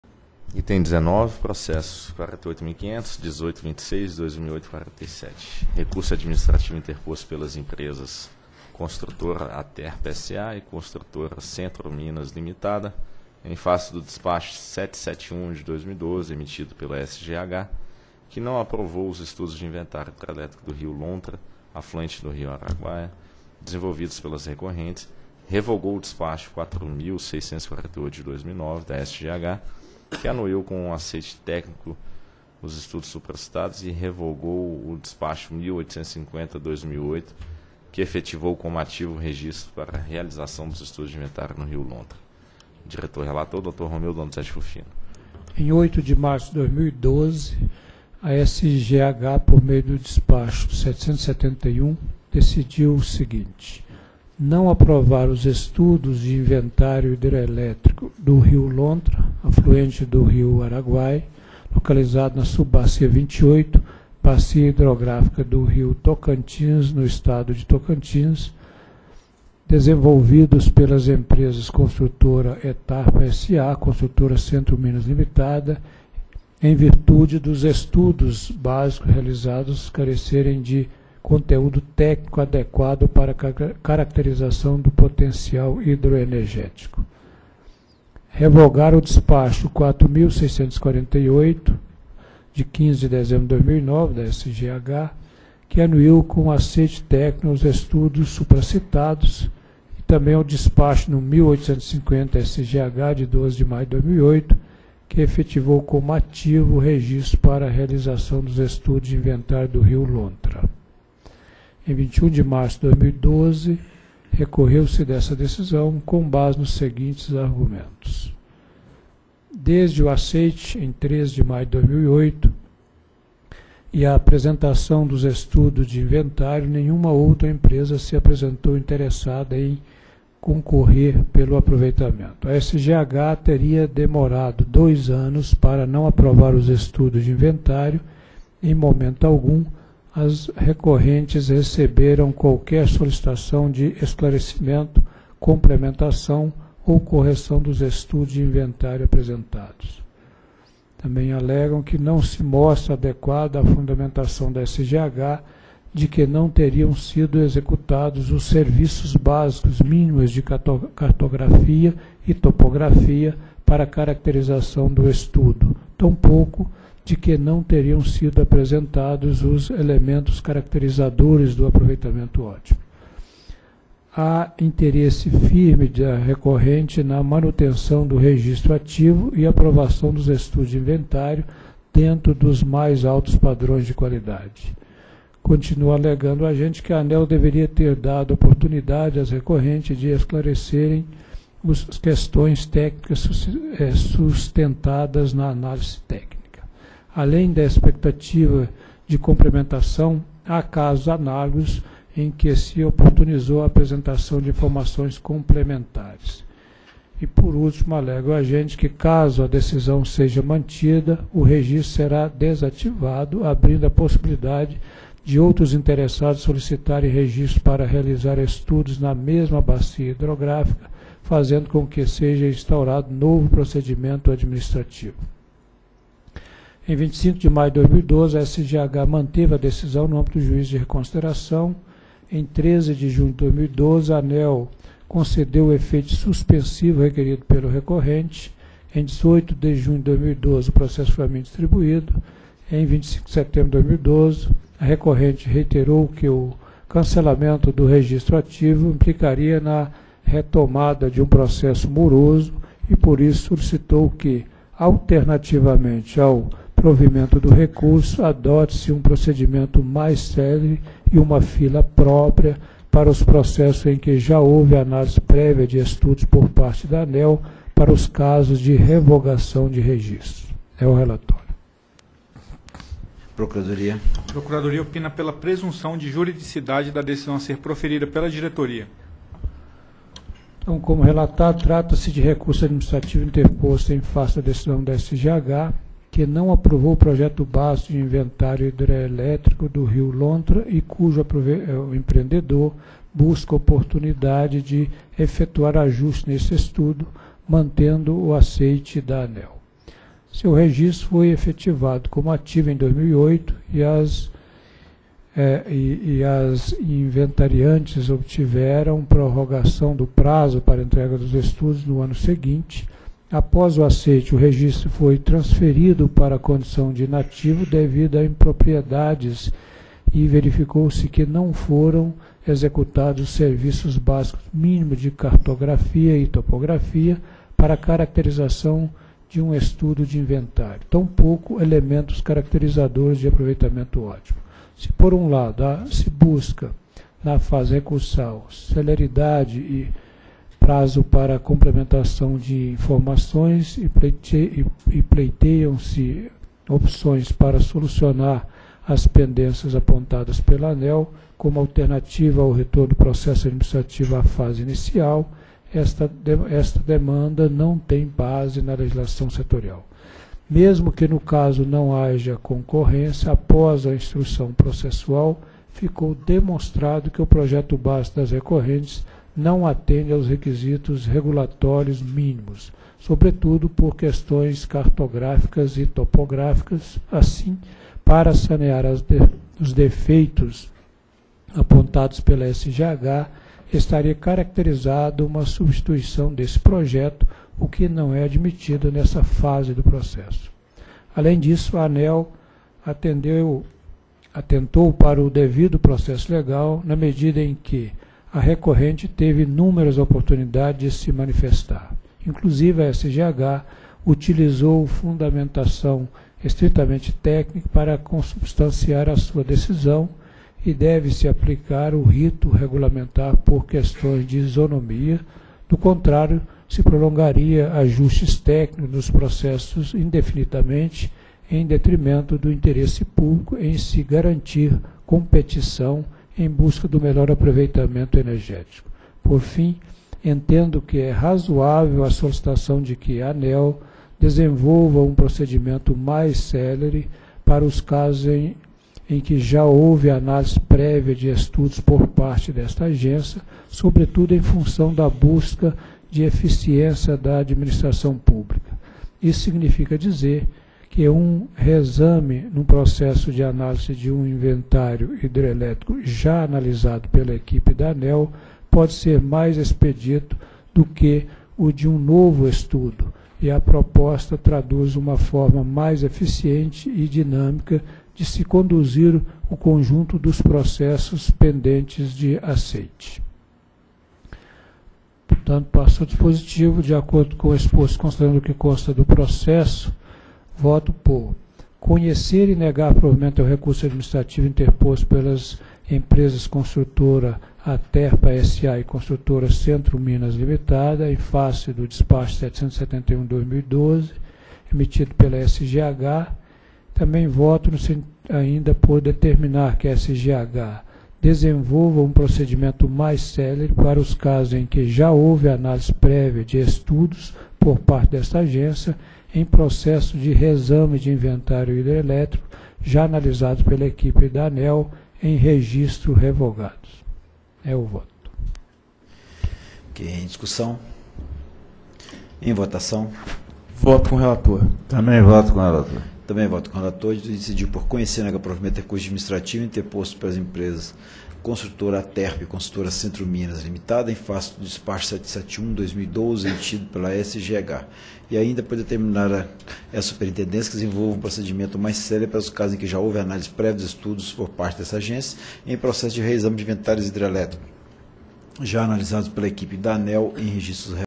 Item 19 - 37ª Reunião Pública Ordinária ANEEL 02/10/2012
Assunto: Recurso Administrativo interposto pelas empresas Construtora Aterpa S.A. e Construtora Centro Minas Ltda. em face do Despacho nº 771/2012, emitido pela Superintendência de Gestão e Estudos Hidroenergéticos -- SGH, que não aprovou os Estudos de Inventário Hidrelétrico do rio Lontra, afluente do rio Araguaia, desenvolvidos pelas Recorrentes, revogou o Despacho nº 4.648/2009-SGH, que anuiu com o aceite técnico os Estudos supracitados e revogou o Despacho nº 1.850/2008-SGH, que efetivou como ativo o registro para a realização dos estudos de inventário do rio Lontra. Relator: Diretor Romeu Donizete Rufino.